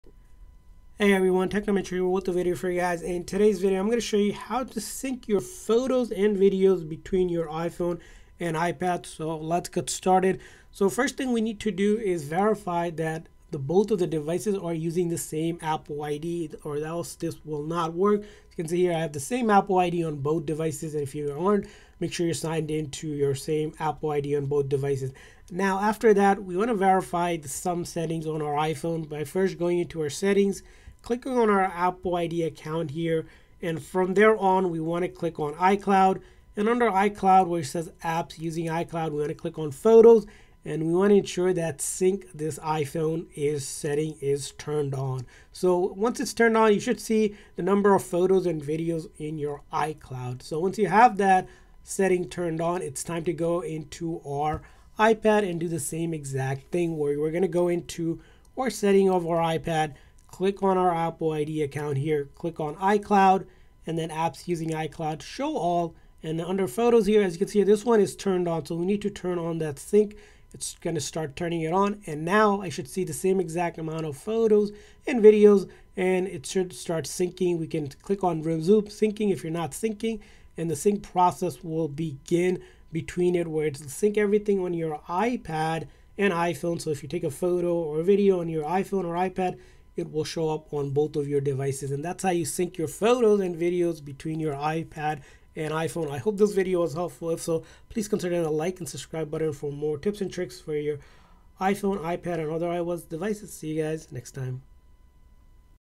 How to Sync Photos on iPad and iPhone
In this iPhone and iPad tutorial you will learn how to sync your photos and videos between your devices using iCloud. Once sync is enabled, photos and videos will appear on both iPad and iPhone at the same time so when you take pictures one device, it will sync to the other device.